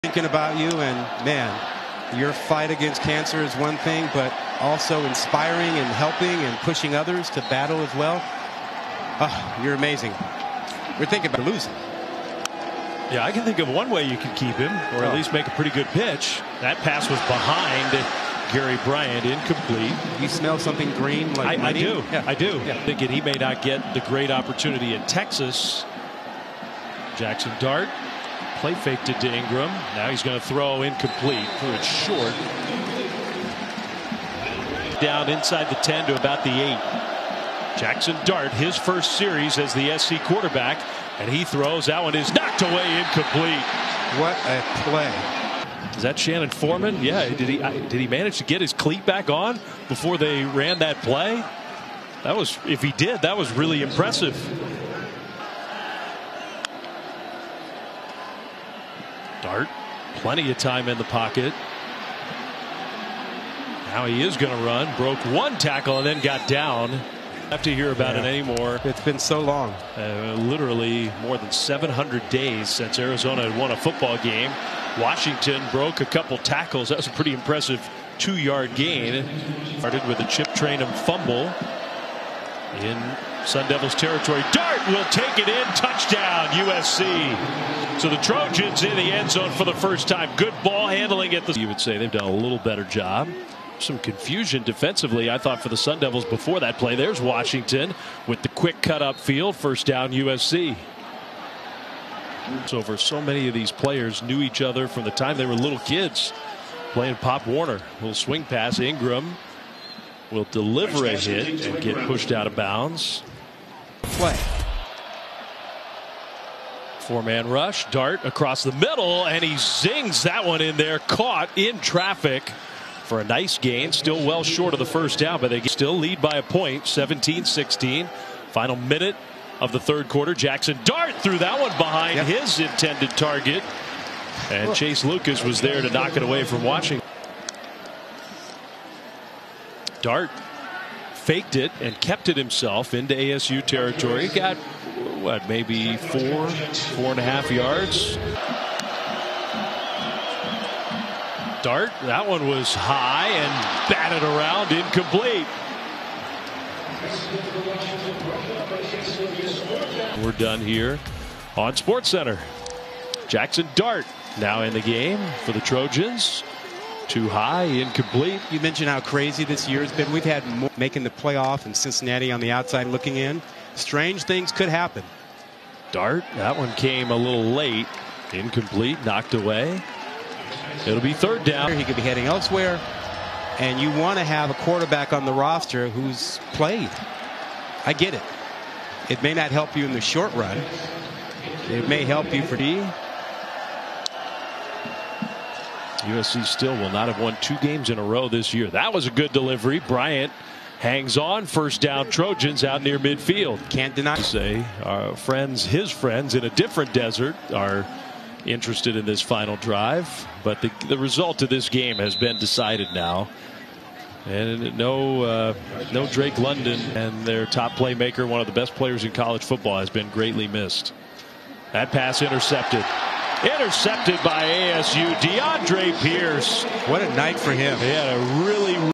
NFL Draft Film Ep 216: Jaxson Dart / QB / USC / 2021 / vs Arizona State
Jaxson Dart #2 QB USC 2021 vs Arizona State